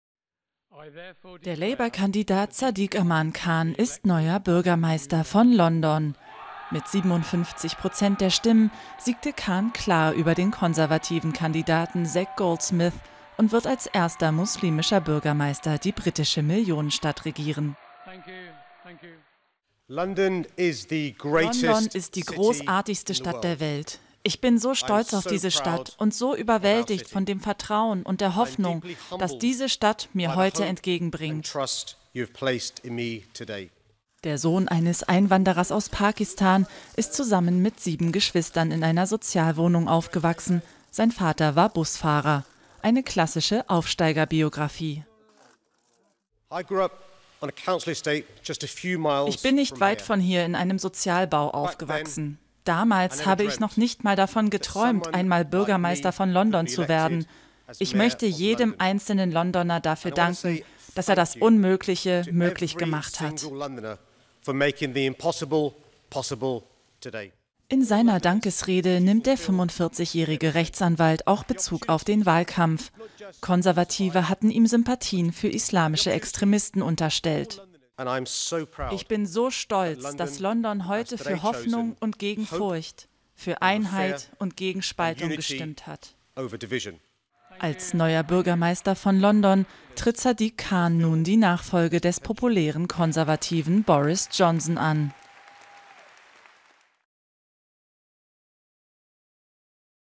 Sadiq Khan: Aus Londons Sozialbauten zum Bürgermeister
London wird künftig erstmals von einem muslimischen Bürgermeister regiert: Der Labour-Kandidat Sadiq Khan, Sohn eines Einwanderers aus Pakistan, siegte klar über den konservativen Kandidaten Zac Goldsmith mit 57 Prozent der Stimmen.